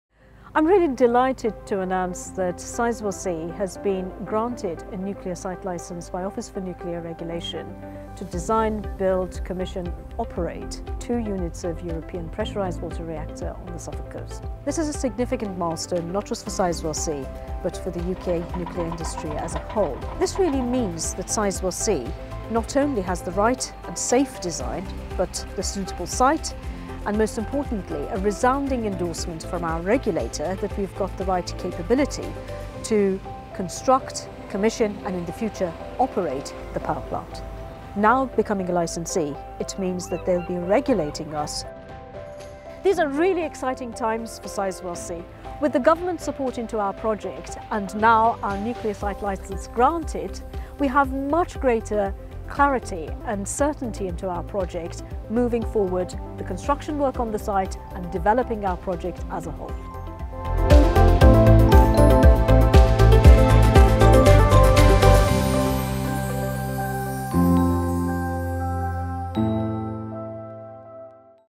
Nuclear Site Licence granted to Sizewell C
Sizewell C has been granted a nuclear site licence, a significant step forward for the new nuclear power project on the Suffolk coast.

It’s the first time that the Office for Nuclear Regulation (ONR) has granted a site licence since issuing one to Hinkley Point C in Somerset in 2012. It comes into force today and follows a rigorous process of scrutiny and assessment by the nuclear regulator.

This milestone reflects the strong momentum behind Sizewell C as we move closer to delivering reliable, low-carbon energy for six million homes.